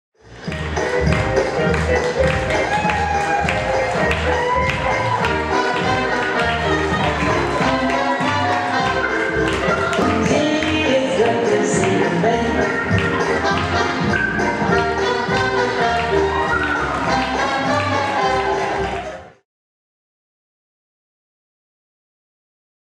TANZSPORT TV - DM SEN I Kombination 2017 - 1. Platz
Die Sieger der Deutschen Meisterschaft Senioren I Kombination 2017:
Frederick Rösler & Janine Rusher (Askania - TSC Berlin)